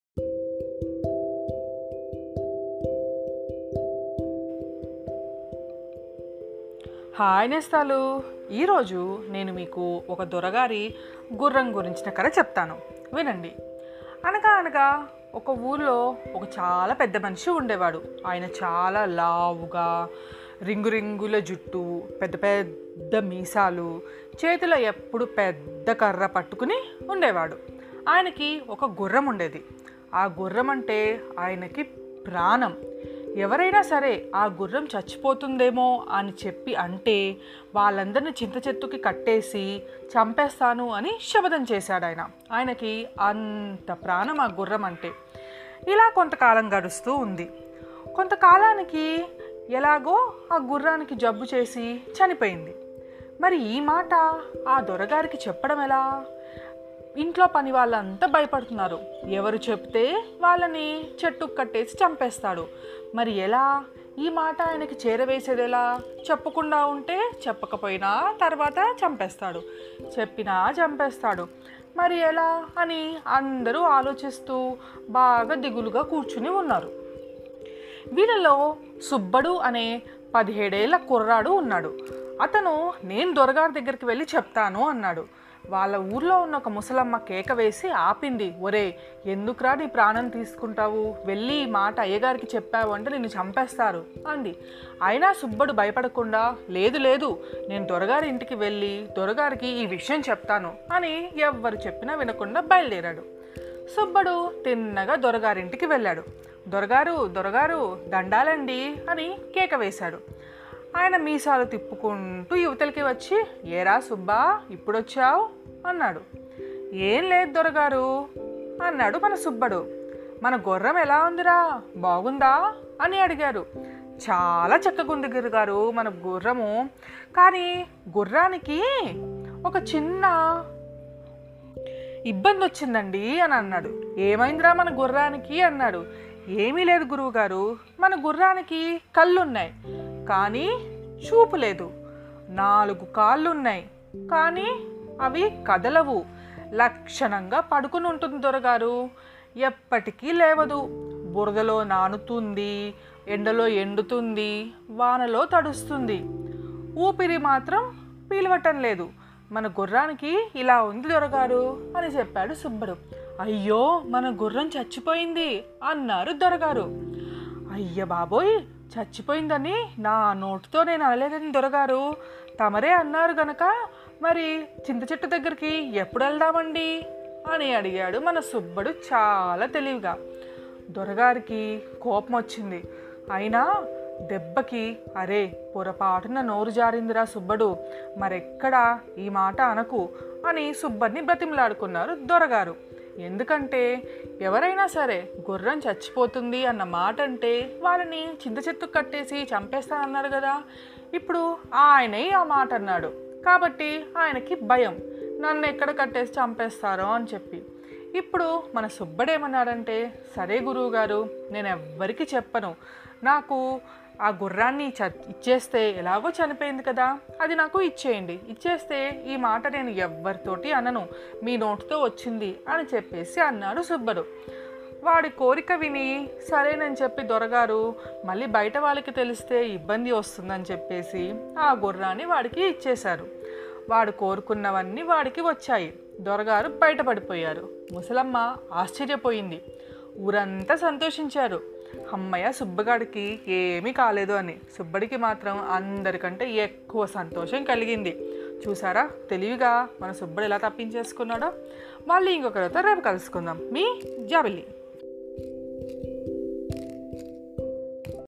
Jaabilli | Doragaari Gurram (దొరగారి గుఱ్ఱం) | Mee Jaabilli | Jaabilli Podcast
In this video I am going to share with you beautiful story from Chandamama Kathalu.